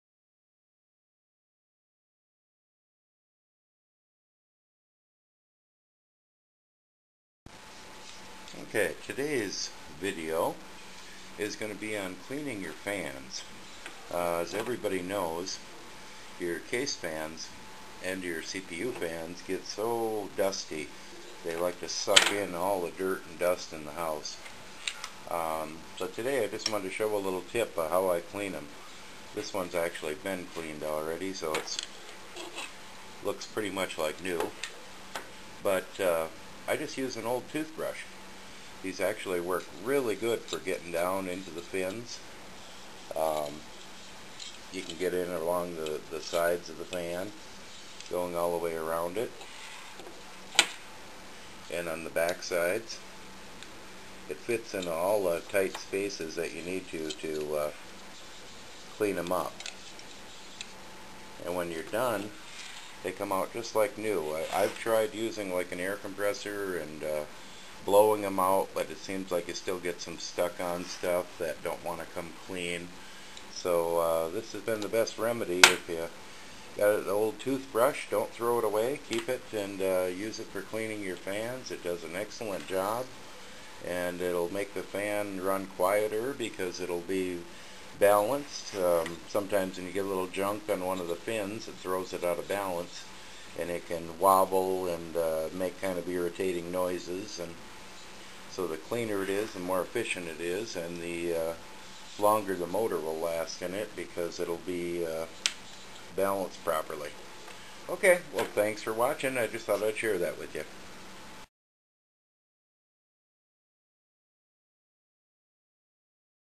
A very simple tip for cleaning all the fans in your computer.
Some thing that i have done for awhile now and thought that would share with every one.